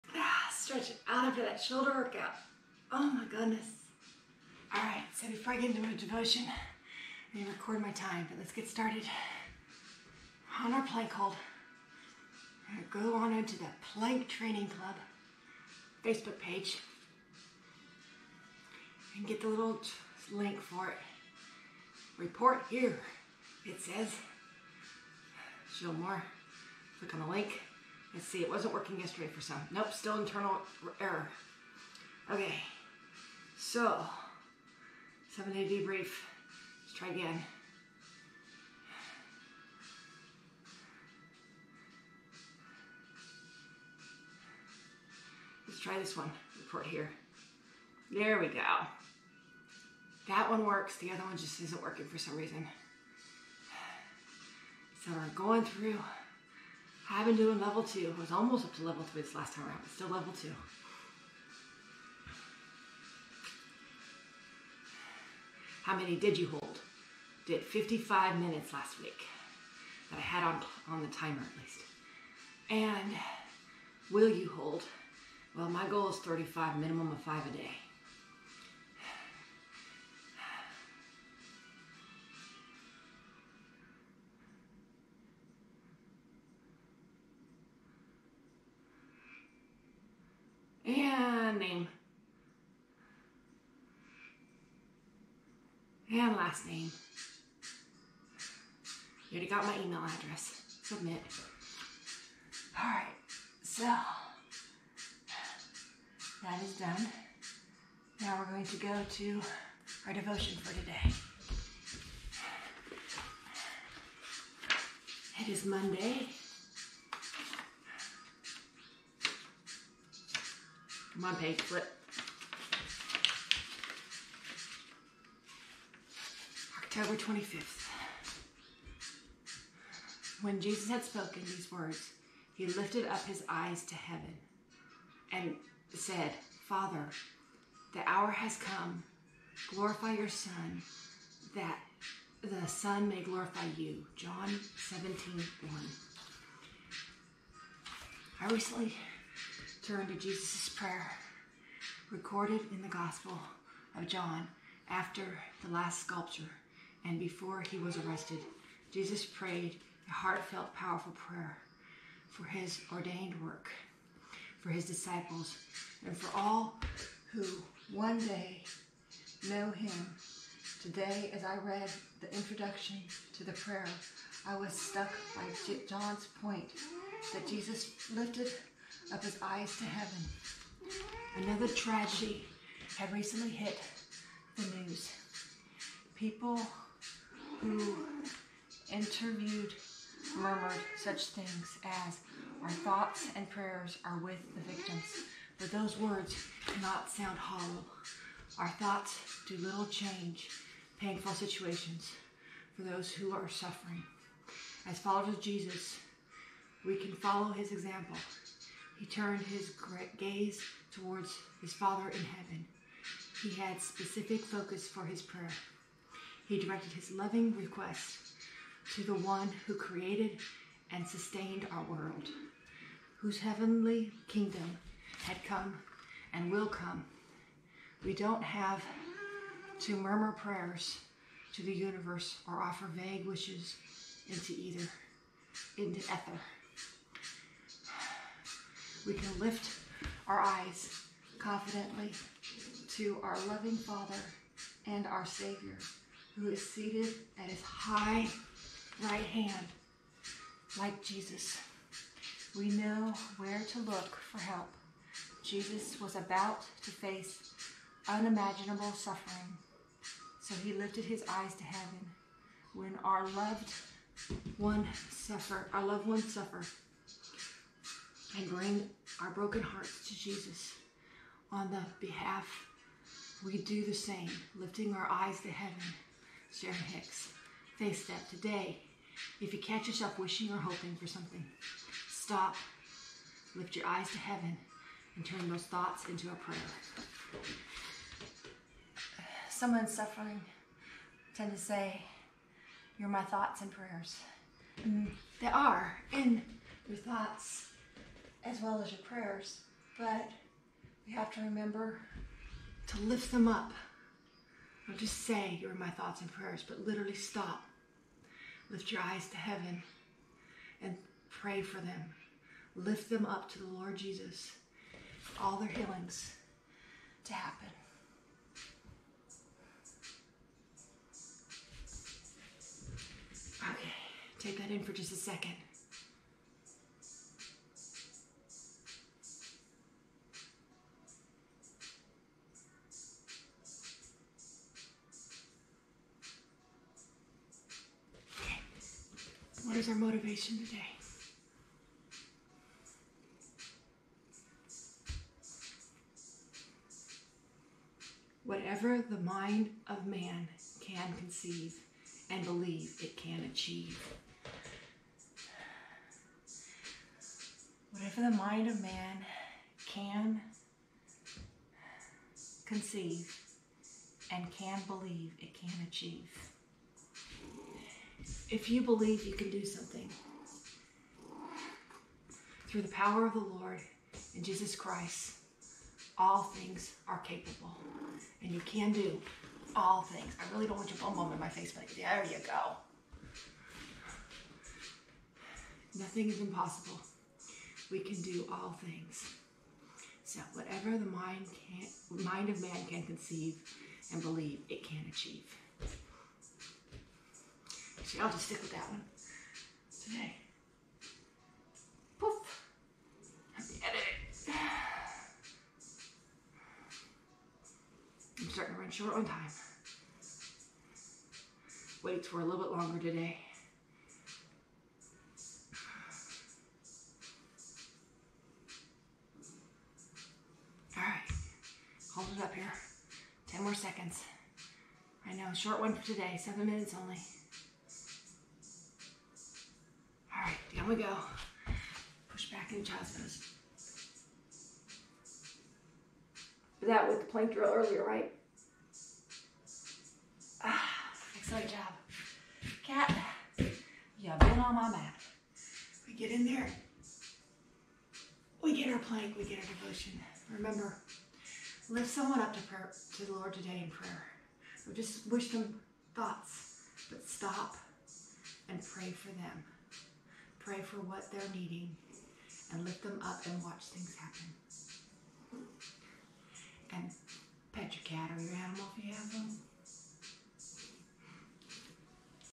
Planks & Devotion - John 17:1
When Jesus had spoken these words, He lifted up His eyes to heaven, and said, "Father, the hour has come; glorify your Son that the Son may glorify you." John 17:1

We don't have to murmur prayers to the universe or offer vague wishes into ether. We can lift our eyes confidently to our loving father and our savior, who is seated at His right hand. Like Jesus, we know where to look for help.

Today, if you catch yourself wishing or hoping for something, stop and lift your eyes to heaven and turn those thoughts into a prayer.